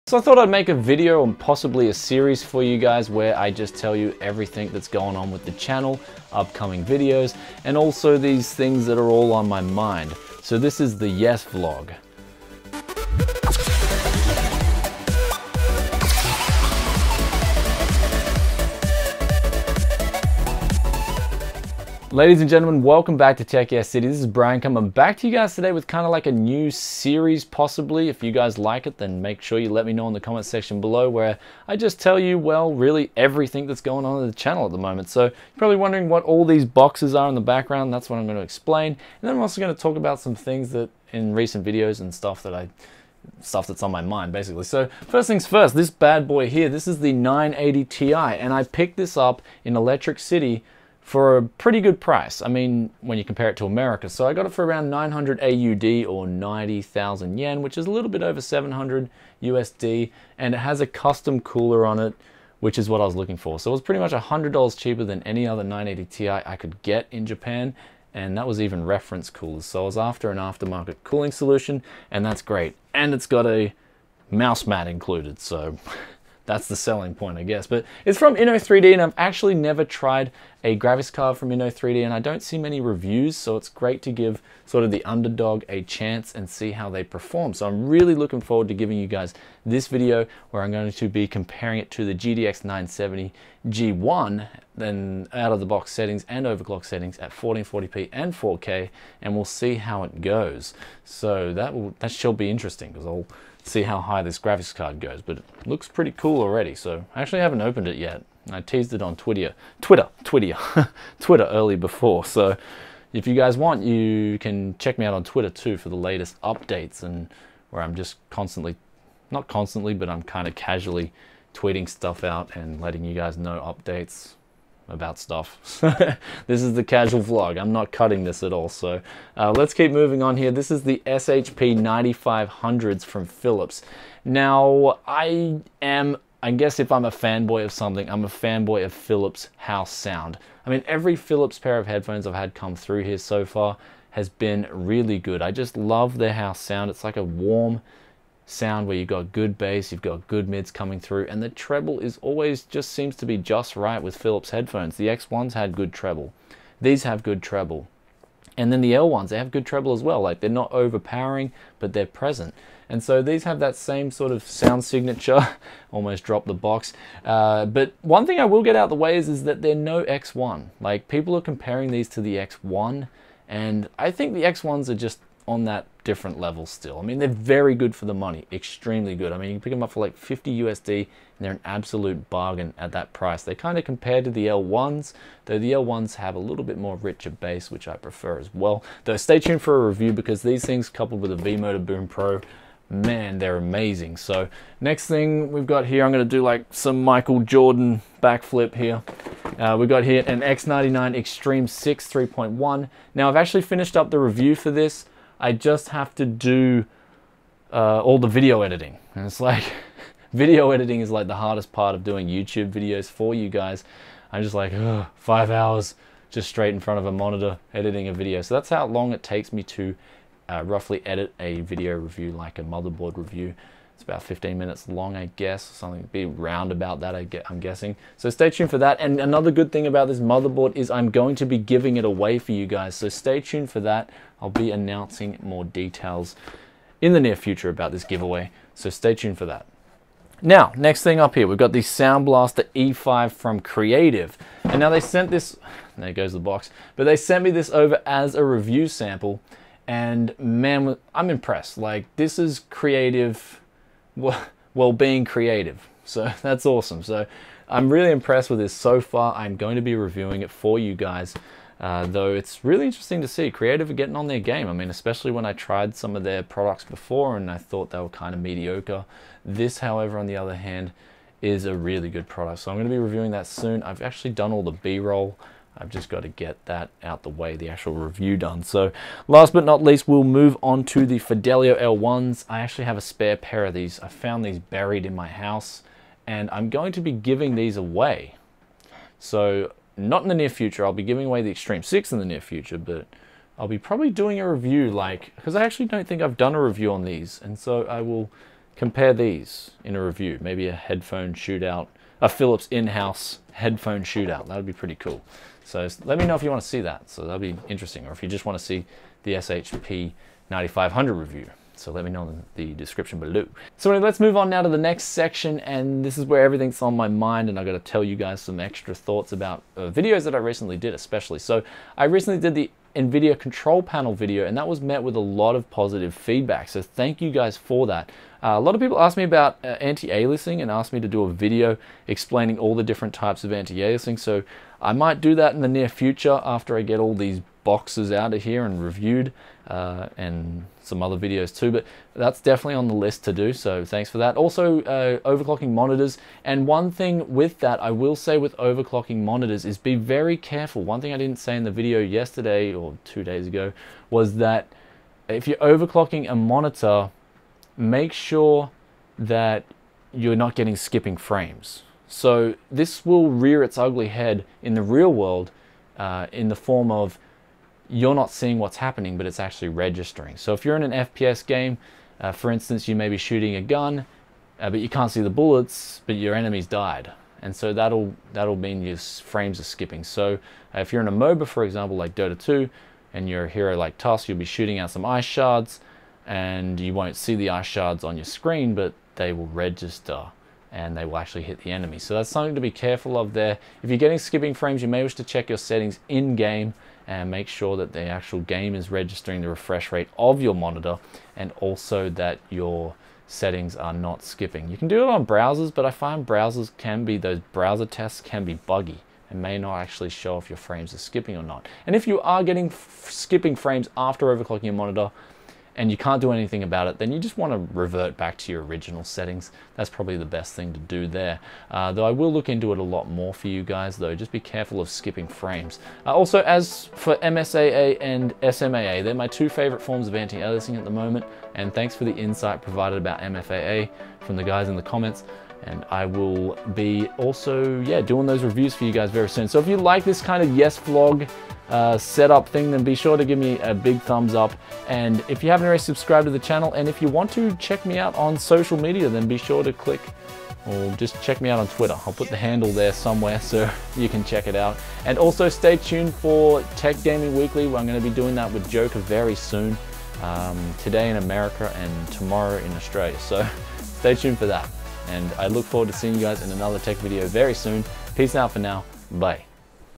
YES Vlog #001 - A New 980ti / SHP9500s / Products to Come / Skipping Frames OC Monitors
With all these new products that have just come in and with the channel starting to get a bit more love, I thought I would well. Give it even more love! So if you guys like this new series then be sure to let me know and I will be sure to make more vlogs for you guys!

*Support TYC for free when you shop on amazon!*